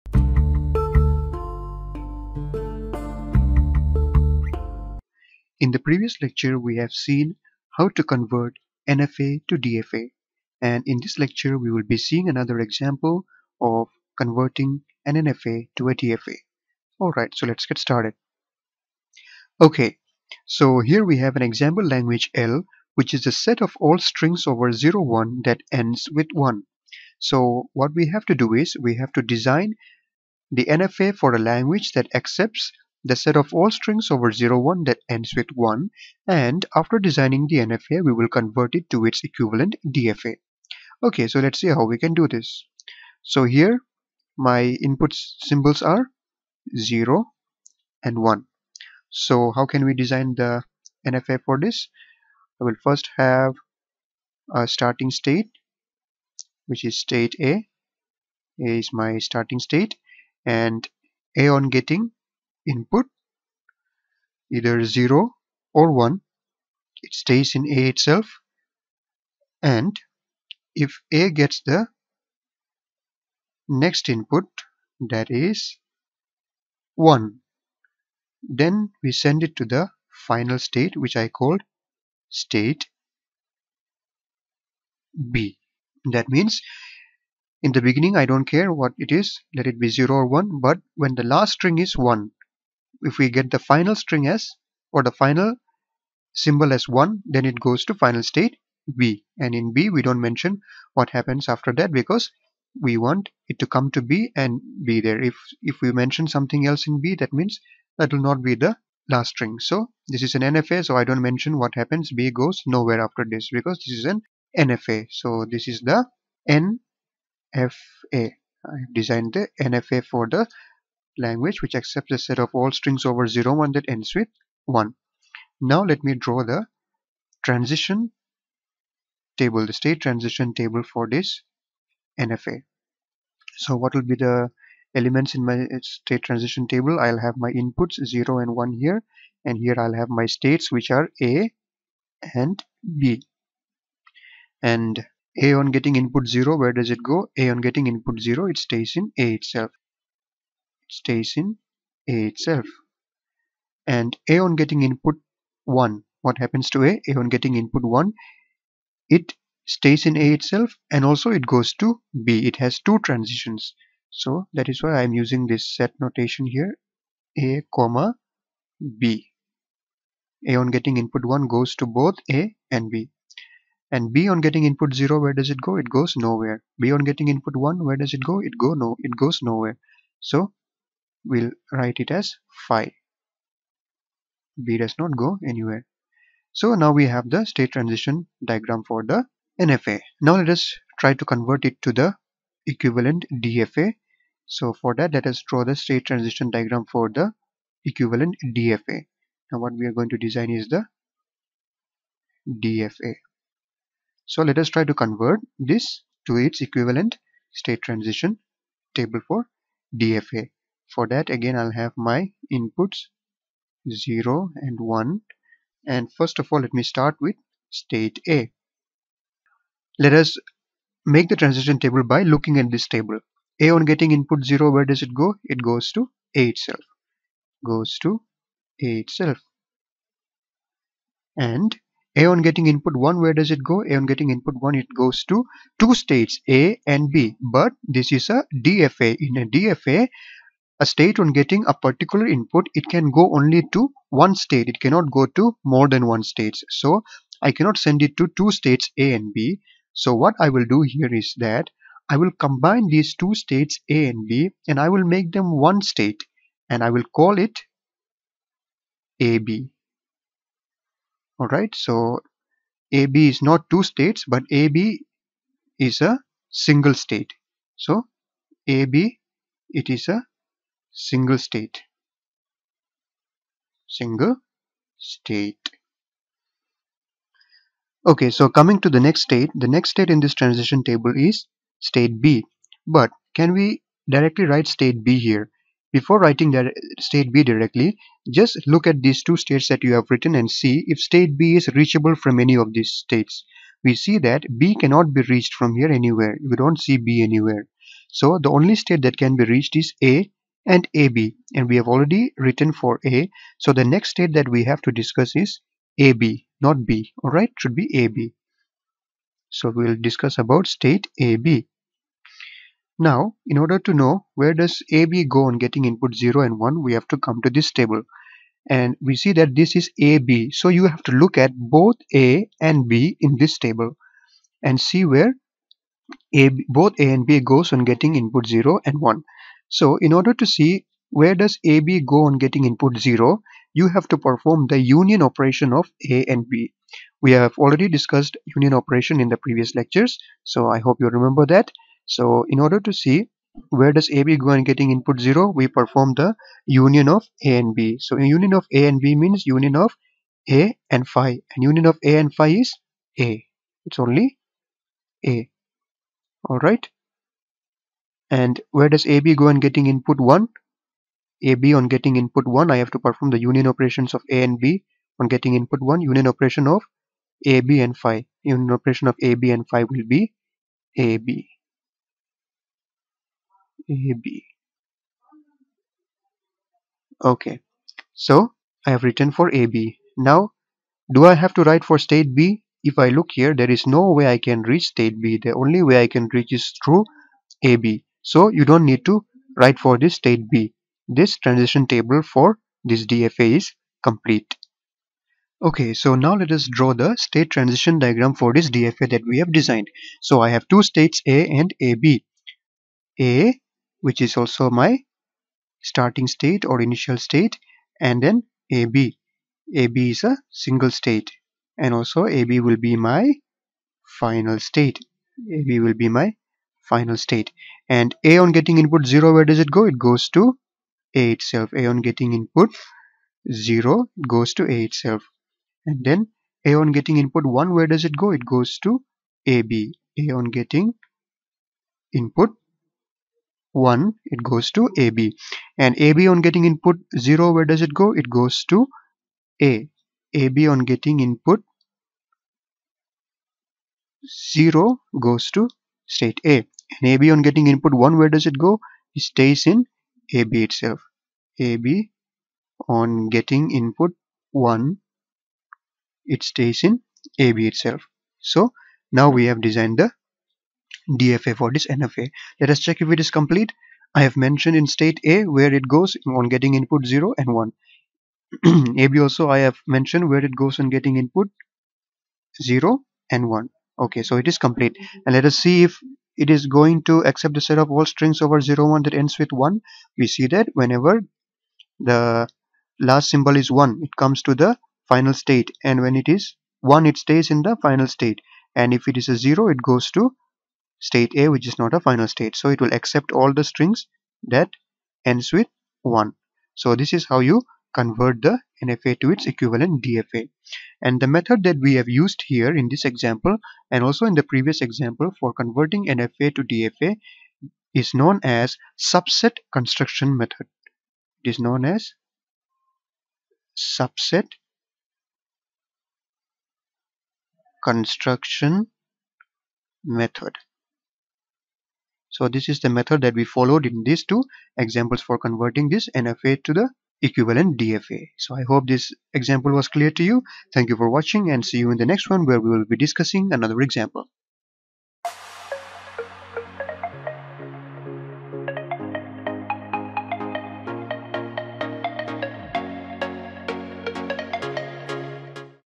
Conversion of NFA to DFA (Example 1)
TOC: Problem Number 1 on Conversion of Non-deterministic Finite Automata (NFA) to Deterministic Finite Automata (DFA).
Topics discussed:
This lecture shows how to convert an NFA that accepts all binary strings over (0,1) that end with '1' to its equivalent DFA using Subset Construction Method.